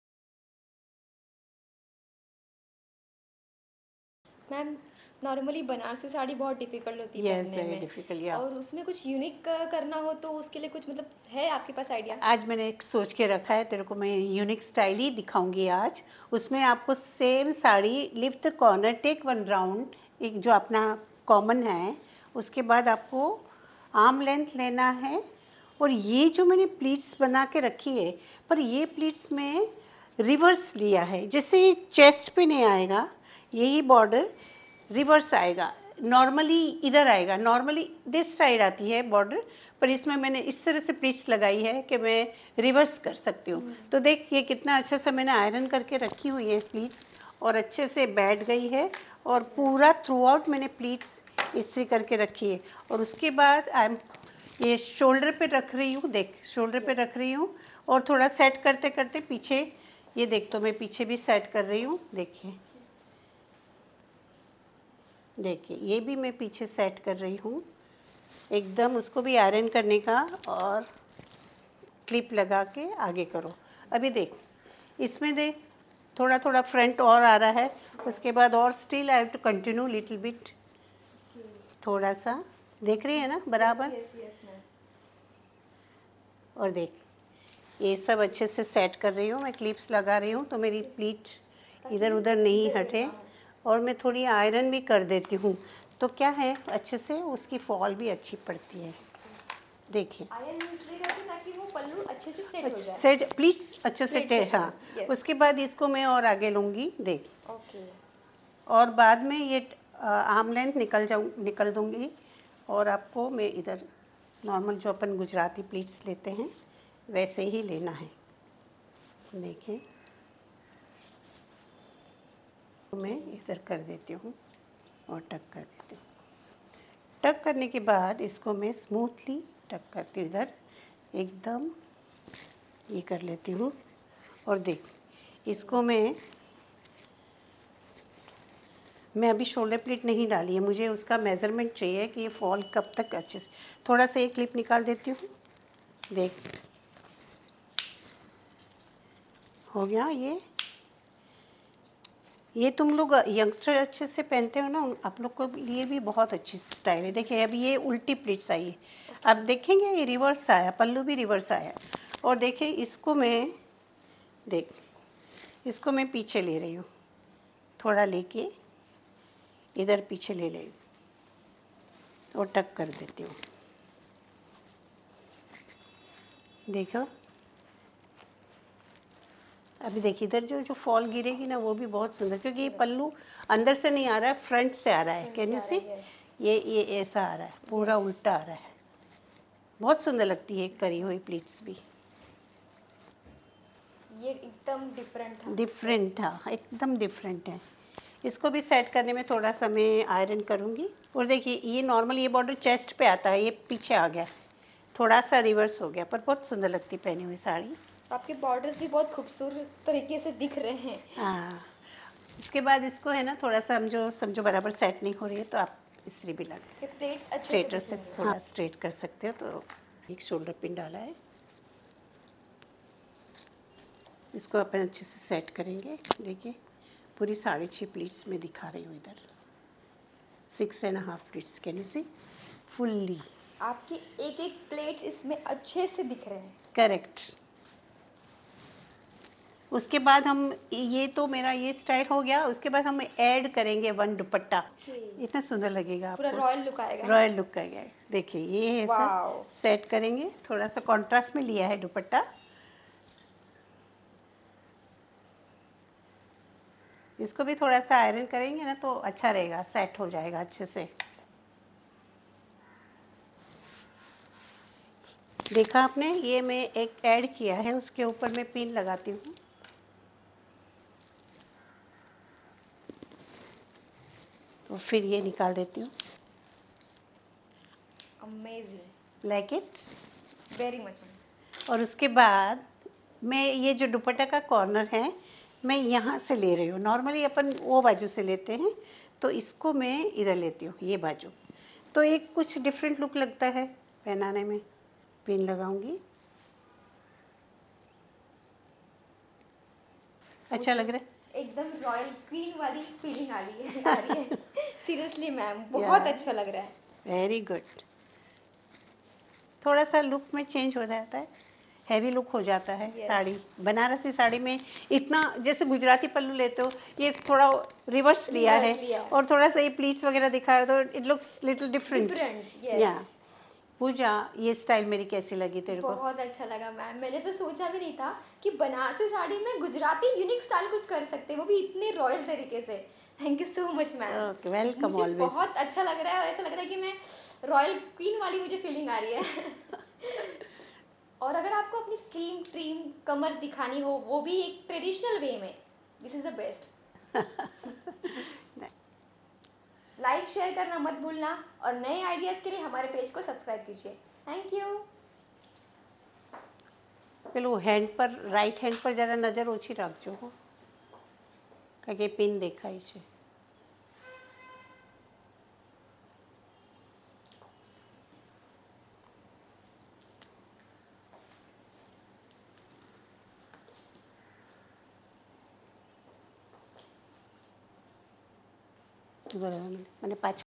Saree No- 148 | Sari Draping Teaching | Kalpana Shah| Banarasi Saree
Learn to drape a Banarasi saree with ease! This guide will help you achieve a royal, queen-like elegance effortlessly. Perfect for special occasions, this traditional style exudes grace and sophistication. Embrace your inner royalty!